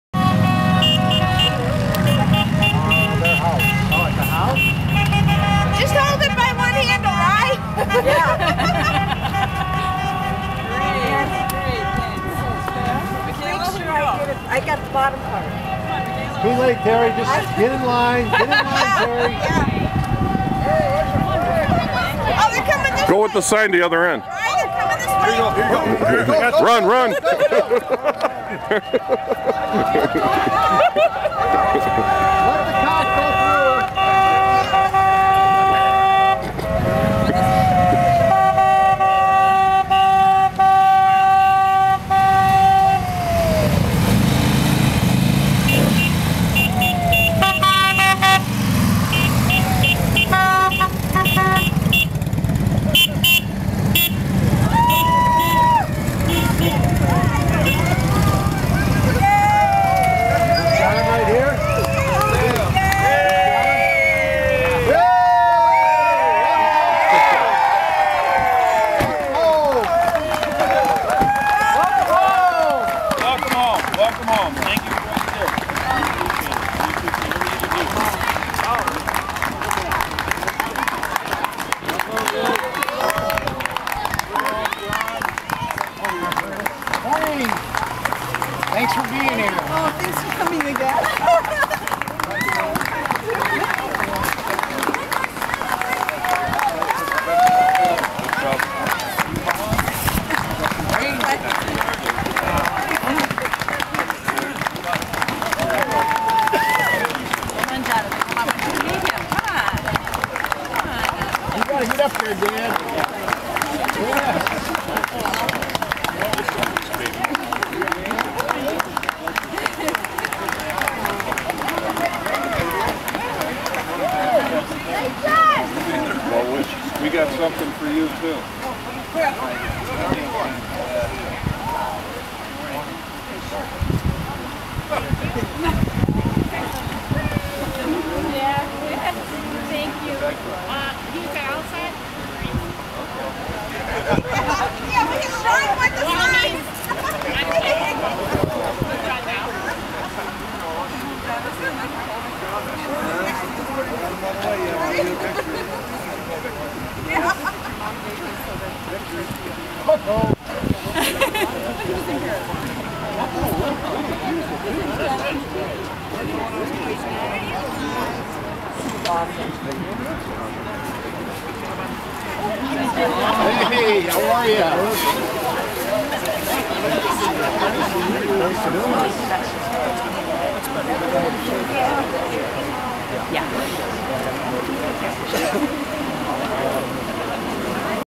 Warriors' Watch Riders Welcome Home Sgt.Jon Dibblee
Illinois Warriors' Watch Riders Welcome Home Sgt. Jon Dibblee.
Sunday, November 23rd, 2008.